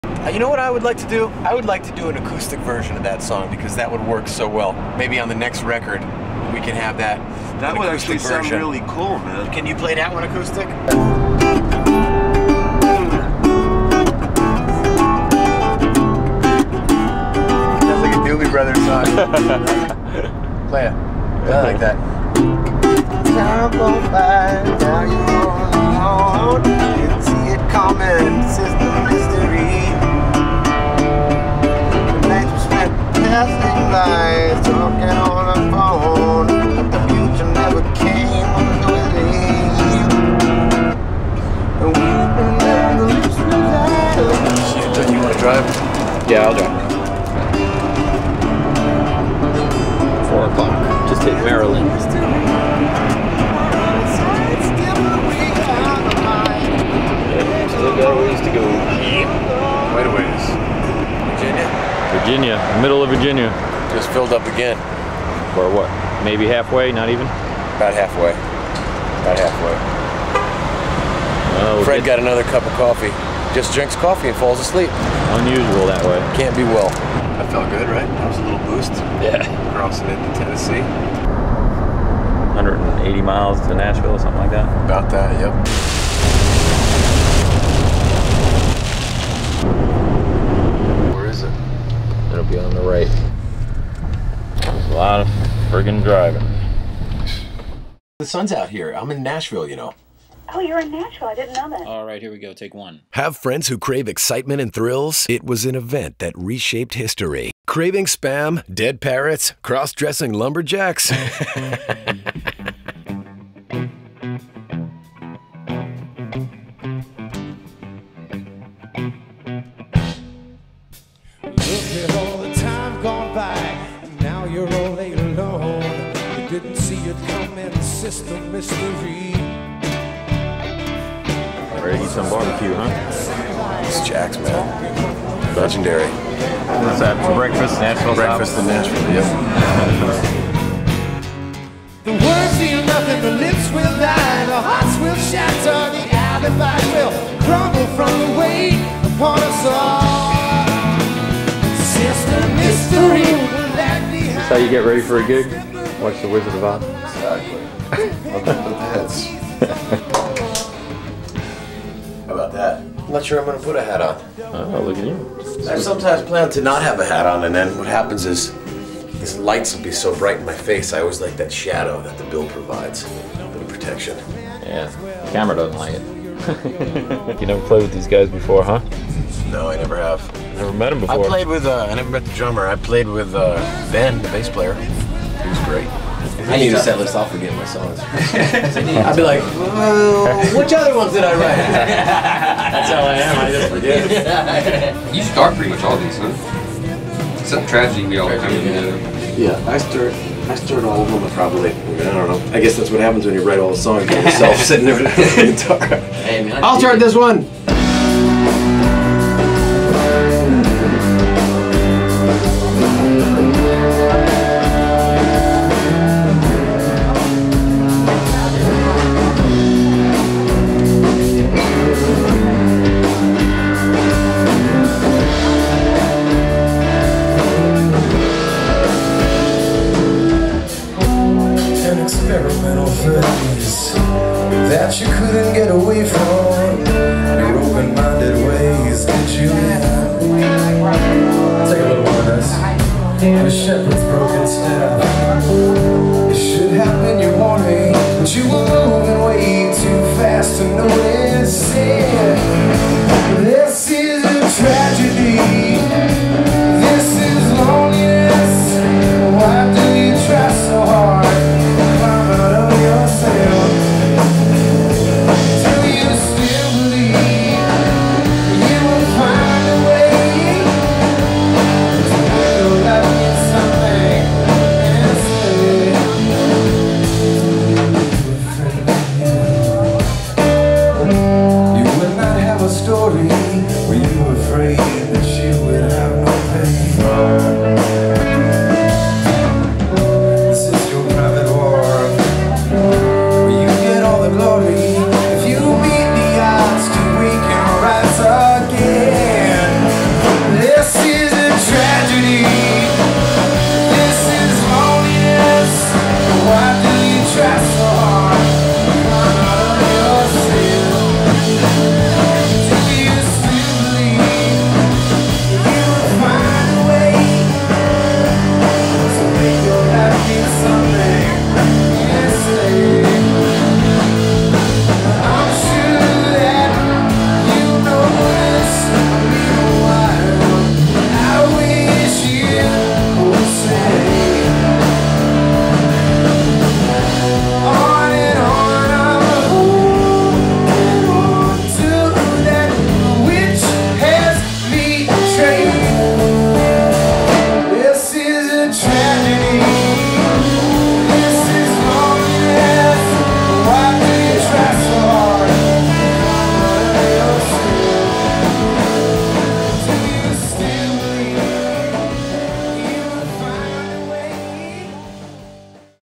Joey DeGraw. Nashville Road Trip. Part 2.
Voice Driven hits the road in this road trip to Nashville Tennessee.  This is Part 2.